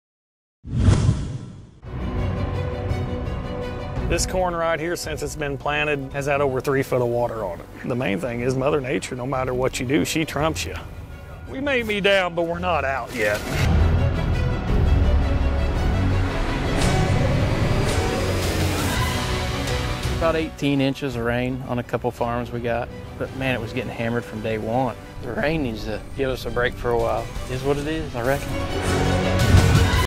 Season 3 - Episode 7 Teaser
Brooks Cardinal and Kevin Kalb are working through the holiday season in hopes of gaining a few extra bushels.  The midwest is still riddled with challenges this year and they have been greatly effected.  Tune in on youtube or our website for the latest content.